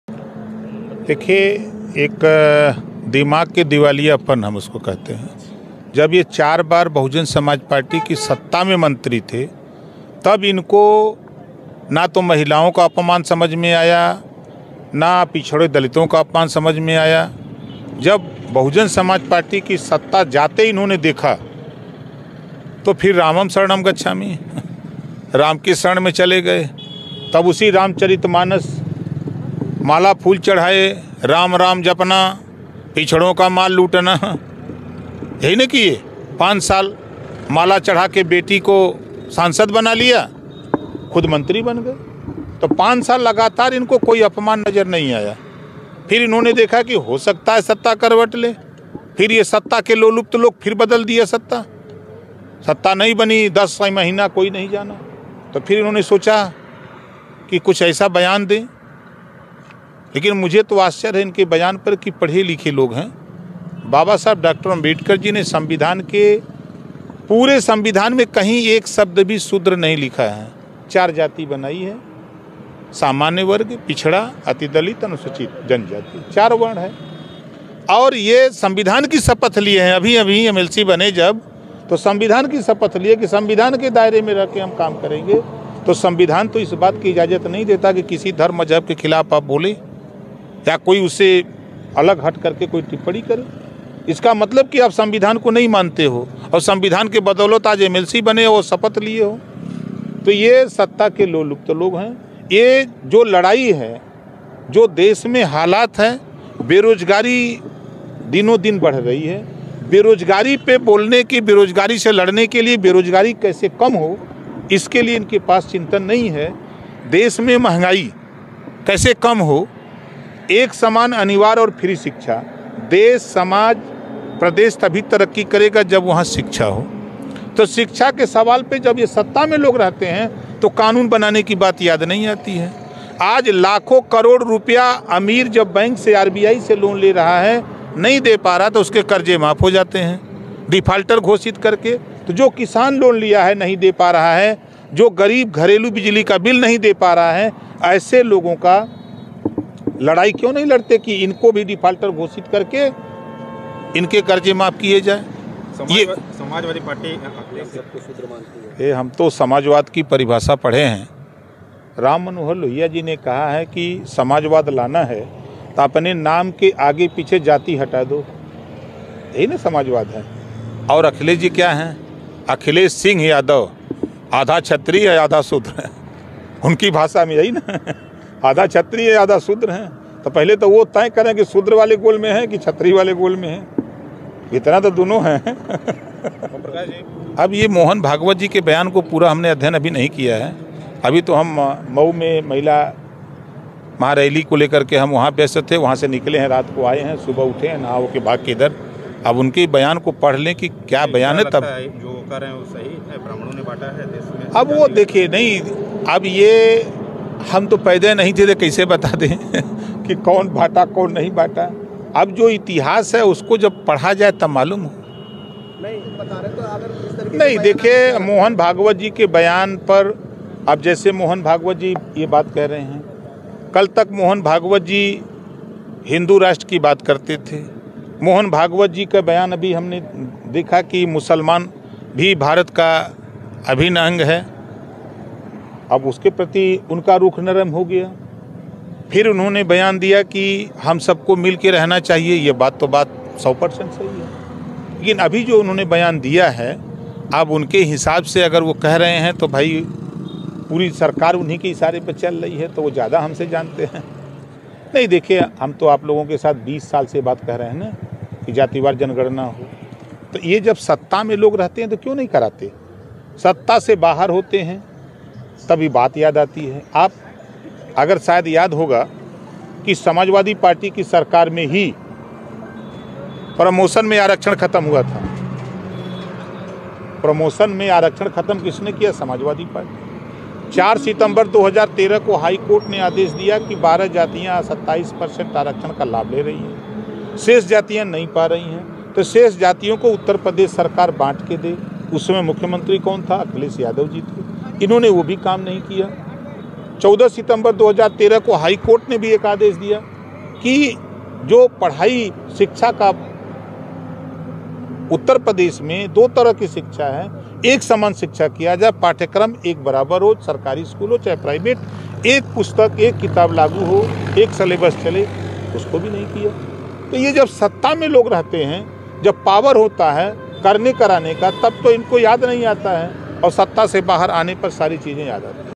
Ramcharitmanas Row: ओपी राजभर बोले- Akhilesh Yadav आधा क्षत्रिय है आधा शूद्र है | Swami Maurya | UP
Ramcharitmanas Row: ओपी राजभर बोले- Akhilesh Yadav आधा क्षत्रिय है आधा शूद्र है, देखें वीडियो..